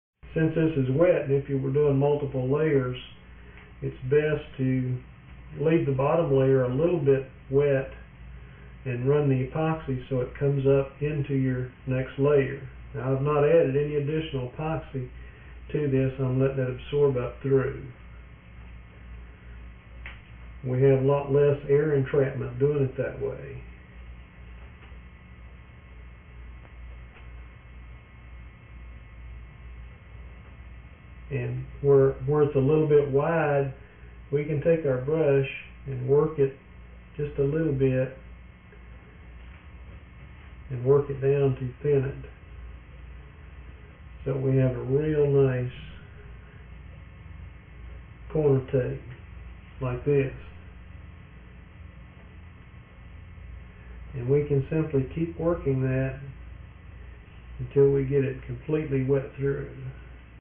Basic Composite Construction Techniques Video, DVD
Basic Composite Construction Techniques is a demonstration of basic fiberglass techniques as applicable for aircraft. Discussions on materials include uni- and bi-directional cloth, resins, flox, micro balloons, foam, core materials, peel ply, etc. Tool use covers brushes squeegees, knives, scales, pumps, hot wire tools, etc. Techniques demonstrated include basic sandwich techniques, corners, edges, and joints, hot wiring, etc. Also included is a basic discussion of setting up your layups to conform to various load bearing areas.
Available for purchase in DVD or eVideo at Aircraft Technical Book Company